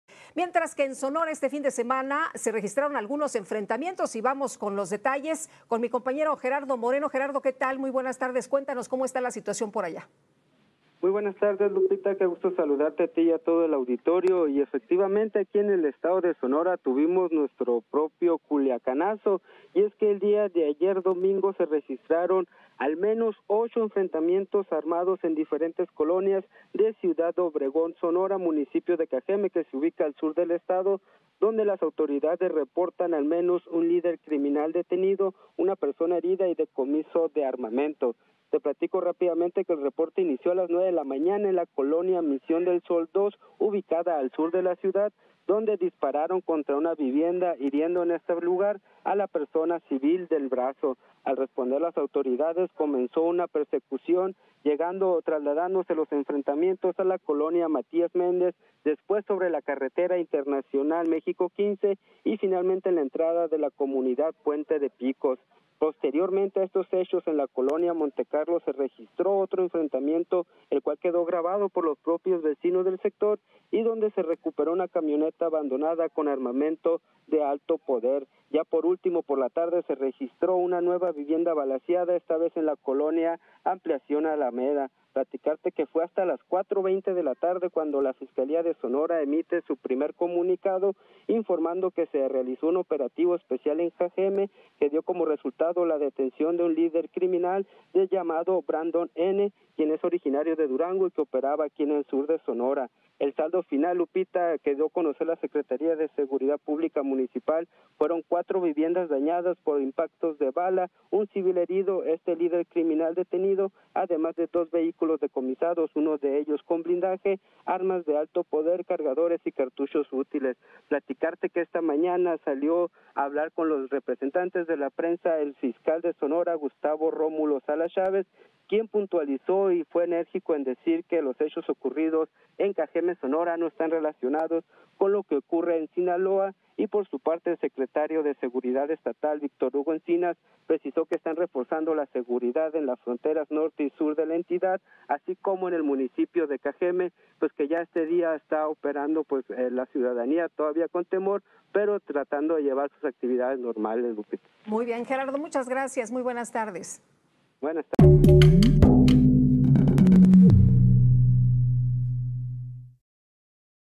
¡Violencia se muda a Sonora! Se registran violentos enfrentamientos en Ciudad Obregón
Este domingo se registraron varios enfrentamientos armados en diferentes colonias de Ciudad Obregón, Sonora, municipio de Cajeme dónde autoridades reportan al menos un líder criminal detenido, además, de varias personas heridas y decomiso de armamento táctico.

Según los reportes de medios locales los hechos iniciaron durante la mañana en la colonia Prados de Tepeyac, dónde presuntamente se privó de la libertad a una persona y eso desencadenó varios enfrentamientos armados.

Luego se registró un fuerte enfrentamiento se suscitó en la colonia Misión del Sol II, dónde dispararon contra una vivienda hiriendo a una persona que fue trasladada a recibir atención medica. Después los hechos se trasladaron a la colonia Matías Méndez, a la Colonia Cajeme y la comunidad de Puente de Picos.